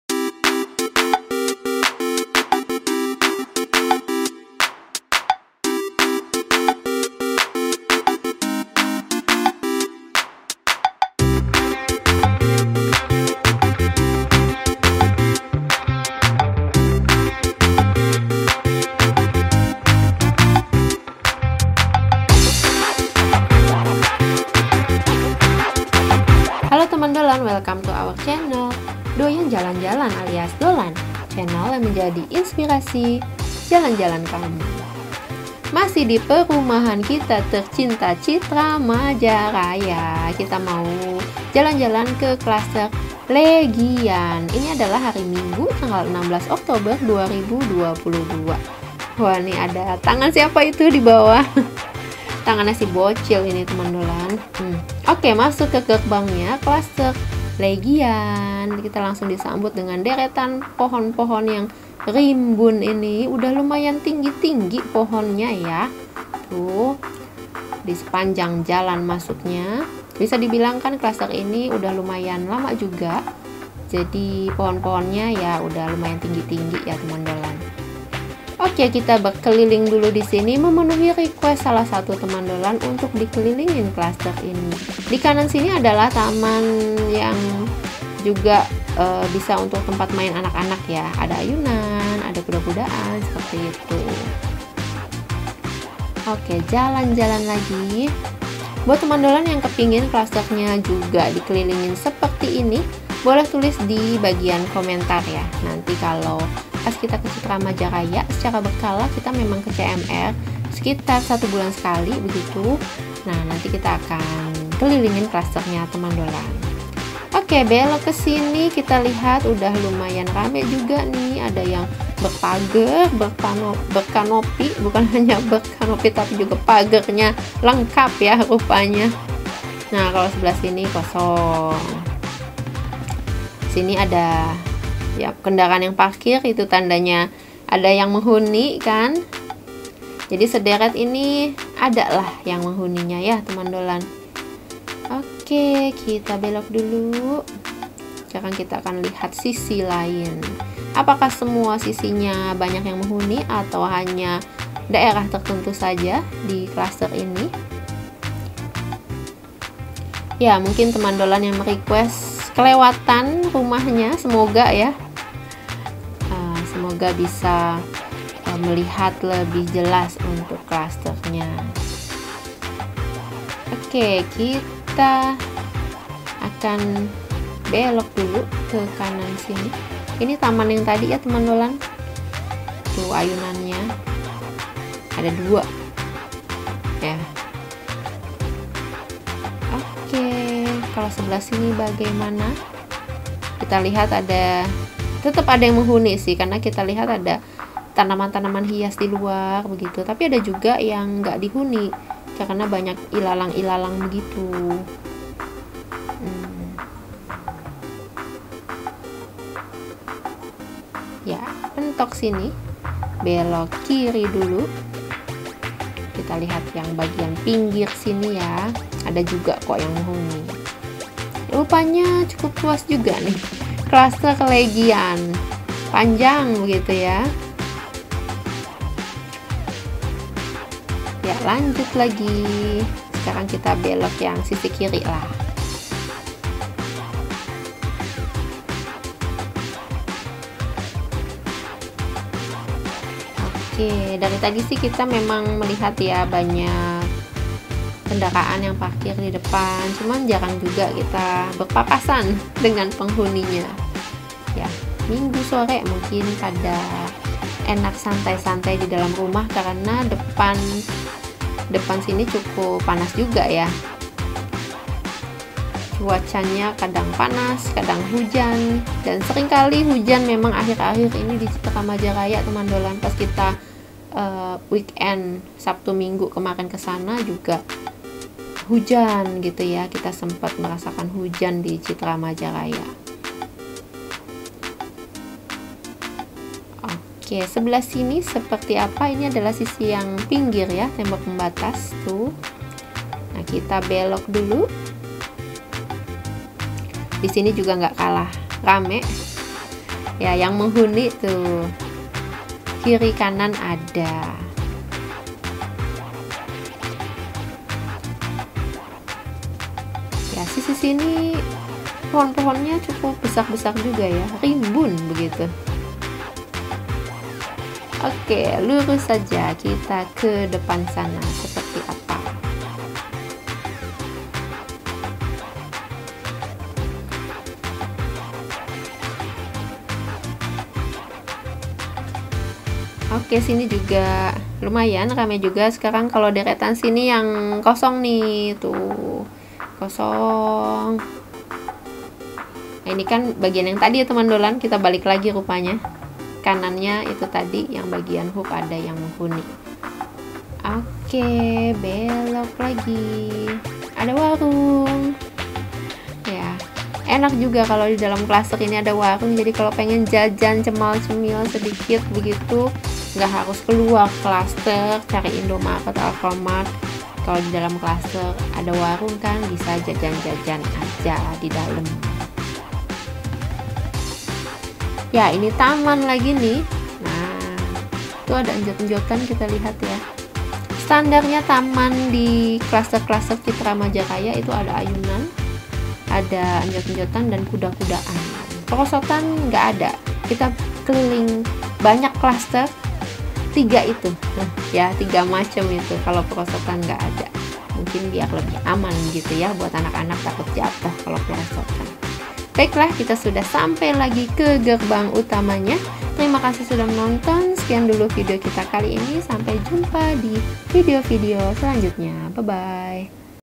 BEGINILAH UPDATE SUASANA CLUSTER LEGIAN CMR 2, 16 OKTOBER 2022. SUDAH ADA WARUNG TAPI MASIH SEPI
Halo teman DOlan ketemu lagi nih, semoga semua dalam keadaan sehat ya.  Kali ini kita mau review kembali cluster Legian CMR 2.  Secara umum sih masih relatif sepi belum ada peningkatan penghuni secara significan ya.  Ada sih rumah yang mulai buka warung.  Semoga yang ambil rumah di cluster ini cepat datang dan nempatin rumah nya ya jadi bisa tambah ramai